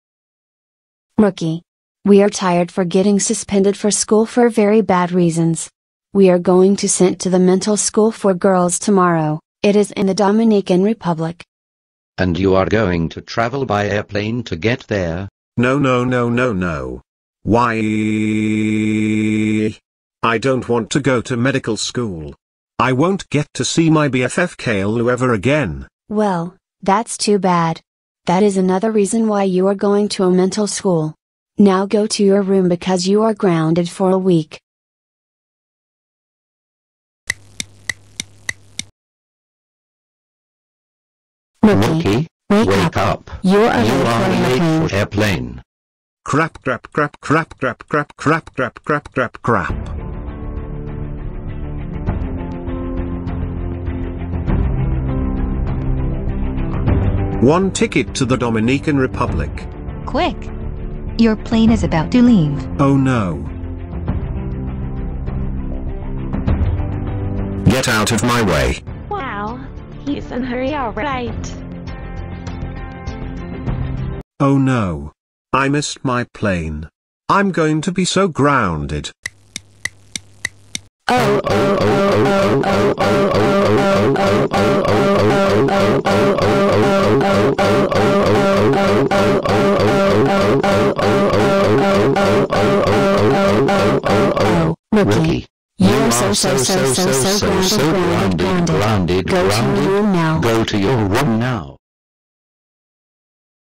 Rookie (Super Mario World) misses his plane/grounded
Inspire by OfficerPoop247